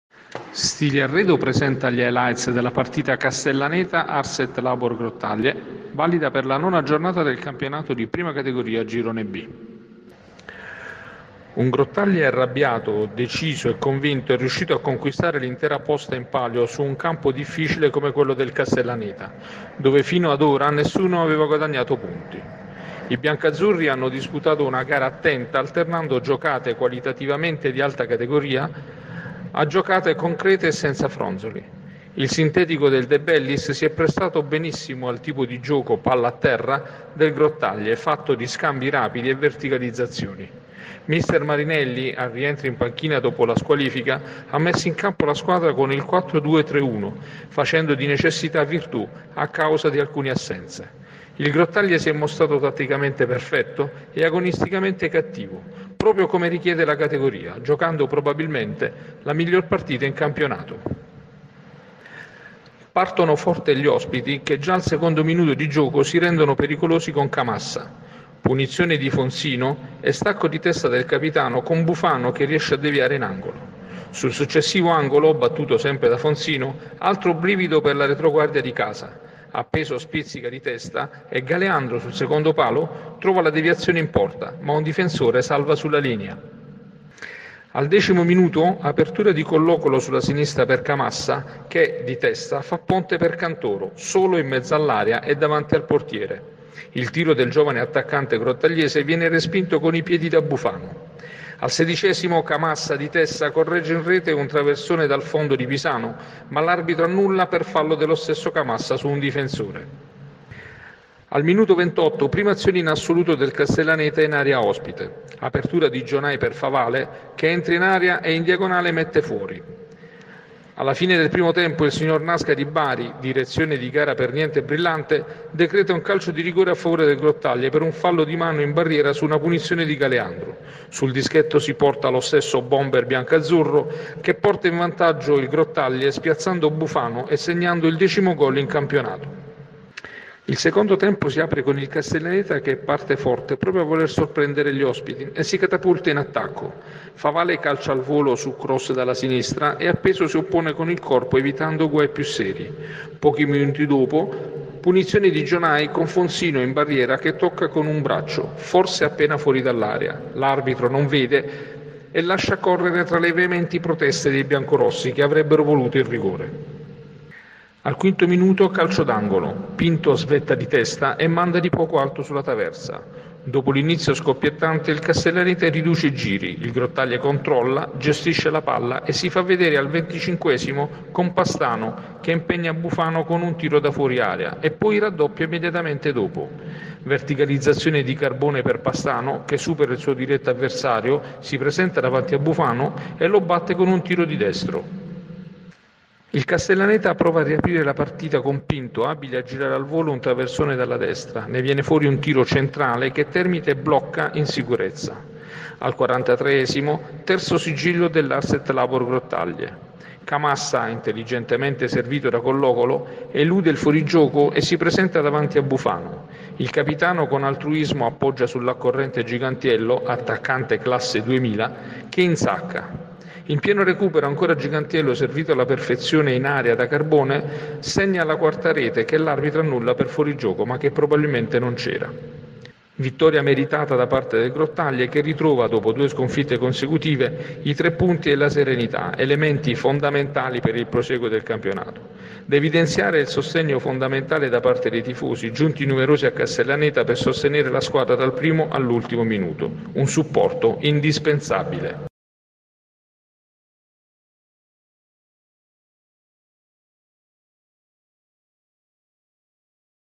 Highlights Castellaneta-Ars et Labor Grottaglie 19/11/2017
Style & Arredo presenta gli highlights della partita Castellaneta – Ars et Labor Grottaglie valida per la nona giornata del campionato di Prima Categoria Girone B
Un Grottaglie arrabbiato, deciso e convinto è riuscito a conquistare l’intera posta in palio su un campo difficile come quello del Castellaneta dove fino ad ora nessuno aveva guadagnato punti. I biancoazzurri hanno disputato una gara attenta alternando giocate qualitativamente di altra categoria a giocate concrete e senza fronzoli. Il sintetico del De Bellis si è prestato benissimo al tipo di gioco, palla a terra, del Grottaglie fatto di scambi rapidi e verticalizzazioni. Mister Marinelli, al rientro in panchina dopo la squalifica, ha messo in campo la squadra con il 4-2-3-1 facendo di necessità virtù causa alcune assenze. Il Grottaglie si è mostrato tatticamente perfetto e agonisticamente cattivo proprio come richiede la categoria giocando, probabilmente, la miglior partita in campionato.
Partono forte gli ospiti che già al secondo minuto di gioco si rendono pericolosi con Camassa. Punizione di Fonzino e stacco di testa del capitano con Bufano che riesce a deviare in angolo. Sul successivo angolo, battuto sempre da Fonzino, altro brivido per la retroguardia di casa, Appeso spizzica di testa e Galeandro, sul secondo palo, trova la deviazione in porta ma un difensore salva sulla linea.
Al decimo minuto apertura di Collocolo sulla sinistra per Camassa che, di testa, fa ponte per Cantoro solo in mezzo all’area e davanti al portiere, il tiro del giovane attaccante grottagliese viene respinto con i piedi da Bufano. Al sedicesimo Camassa di testa corregge in rete un traversone dal fondo di Pisano ma l’arbitro annulla per fallo dello stesso Camassa su un difensore.
Al minuto 28 prima azione in assoluto del Castellaneta in area ospite, apertura di Gjonaj per Favale che entra in area e in diagonale mette fuori. Alla fine del primo tempo il signor Nasca di Bari, direzione di gara per niente brillante, decreta un calcio di rigore a favore del Grottaglie per un fallo di mano in barriera su una punizione di Galeandro. Sul dischetto si porta lo stesso bomber biancoazzurro che porta in vantaggio il Grottaglie spiazzando Bufano e segnando il decimo goal in campionato.
Il secondo tempo si apre con il Castellaneta che parte forte proprio a voler sorprendere gli ospiti e si catapulta in attacco, Favale calcia al volo su cross dalla sinistra e Appeso si oppone con il corpo evitando guai più seri. Pochi minuti dopo punizione di Gjonaj con Fonzino, in barriera, che tocca con un braccio forse appena fuori dell’area, l’arbitro non vede nulla e lascia correre tra le veementi proteste dei biancorossi che avrebbero voluto il rigore.
Al quinto minuto, calcio d’angolo Pinto svetta di testa e manda di poco alto sulla traversa. Dopo l’inizio scoppiettante il Castellaneta riduce i giri, il Grottaglie controlla, gestisce la palla e si fa vedere al 25 con Pastano che impegna Bufano con un tiro da fuori area e poi raddoppia immediatamente dopo. Verticalizzazione di Carbone per Pastano che supera il suo diretto avversario, si presenta davanti a Bufano e lo batte con un tiro di destro.
Il Castellaneta prova a riaprire la partita con Pinto abile a girare al volo un traversone dalla destra, ne viene fuori un tiro centrale che Termite blocca in sicurezza. Al 43° terzo sigillo dell’Ars et Labor Grottaglie. Camassa, intelligentemente servito da Collocolo, elude il fuorigioco e si presenta davanti a Bufano, il Capitano con altruismo appoggia sull’accorrente Gigantiello, attaccante classe 2000, che insacca. In pieno recupero ancora Gigantiello, servito alla perfezione in area da Carbone, segna la quarta rete che l’arbitro annulla per fuorigioco ma che probabilmente non c’era.
Vittoria meritata da parte del Grottaglie che ritrova, dopo due sconfitte consecutive, i tre punti e la serenità elementi fondamentali per il prosieguo del campionato. Da evidenziare il sostegno fondamentale da parte dei tifosi giunti numerosi a Castellaneta per sostenere la squadra dal primo all’ultimo minuto, un supporto indispensabile.